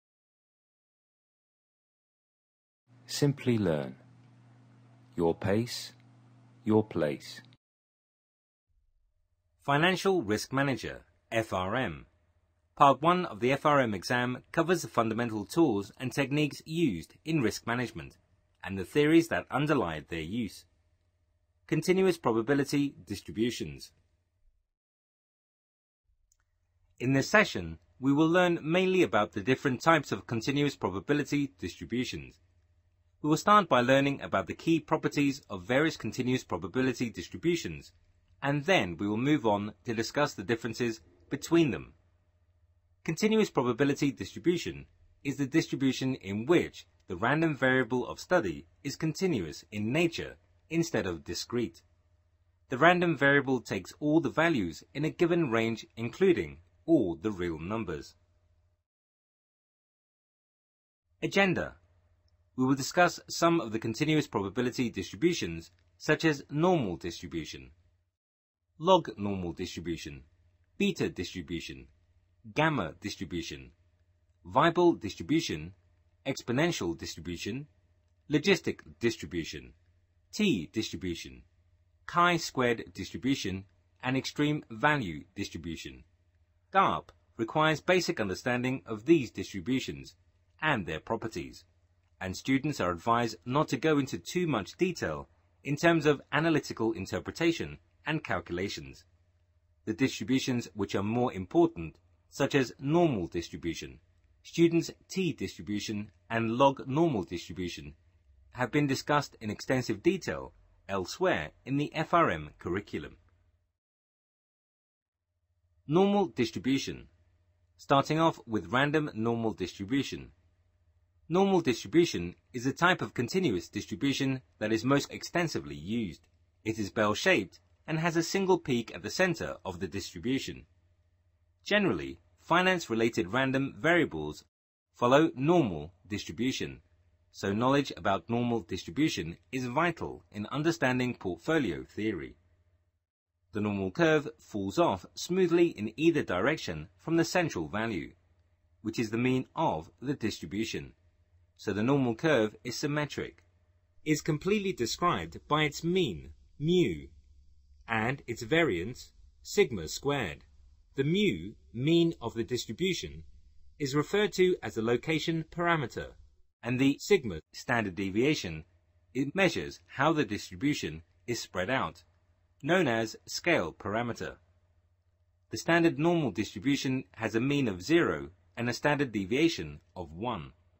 What is Continuous Probability Distributions | Financial Risk Manager Training | Simplilearn
This video explains the:
1.Continuous Probability Distribution covers Normal
2.Exponential
3.Long-Normal
4.Logistic
5.Extreme Value
6.Weibull
7.Gamma
8.Beta
9.Chi-Squared and Student's T Distribution